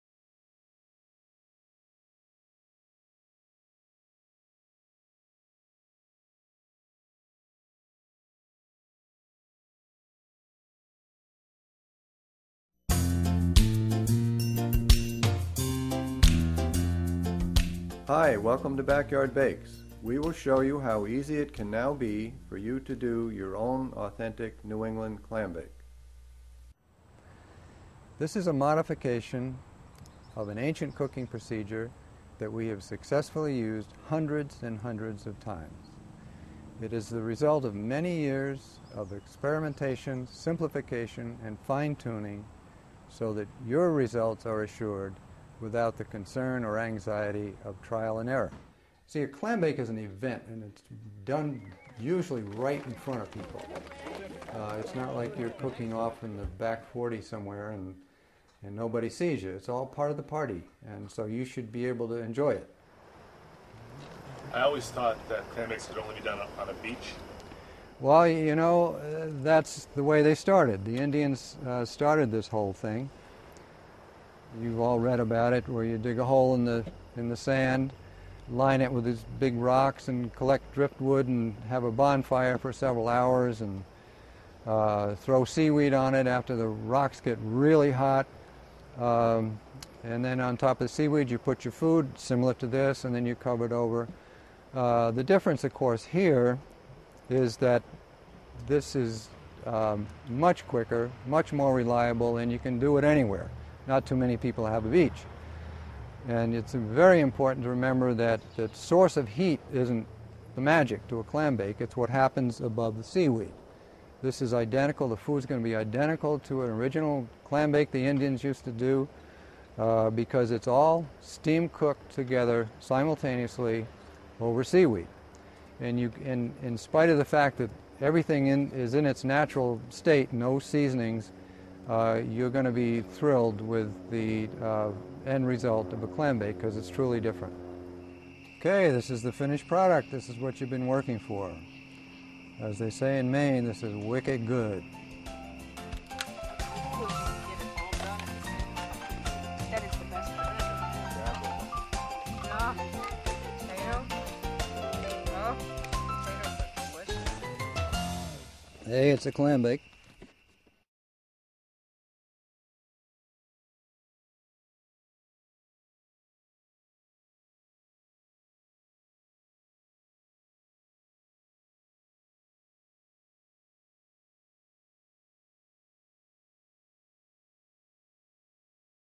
AUTHENTIC CLAMBAKE COOKER SYSTEM
SIMPLE, CONSISTENT, RELIABLE, AND DELICIOUS  - EVERY SINGLE TIME. WITH OVER 30 YEARS
OF EXPERIENCE IN AUTHENTIC NEW ENGLAND CLAMBAKES, WE WILL SHOW YOU, IN DETAIL, HOW YOU CAN QUICKLY AND EASILY BECOME YOUR OWN BAKEMASTER. FOR MORE INFORMATION ABOUT THIS CLAMBAKE SYSTEM VISIT OUR